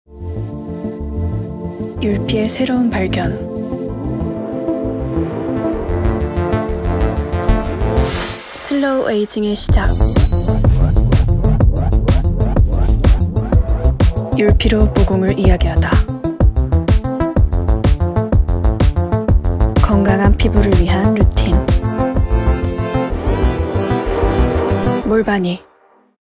[DAZED FILM] NEW FRAME
NEW FRAME

새로운 틀 ‘몰바니’ 속 고민시.
Go Minsi in the new frame ‘Molvany’.

Editor Hyun Junghwan
Fashion Lee Jungju
Photography Kim Yeoungjun
Art Lee Seyeon
Film Na Seunggwon
Hair Hanbyeol
Makeup Oh Yunhee
Set Zeon Suin

더 많은 화보와 기사는 데이즈드 6월호에서 만나볼 수 있습니다.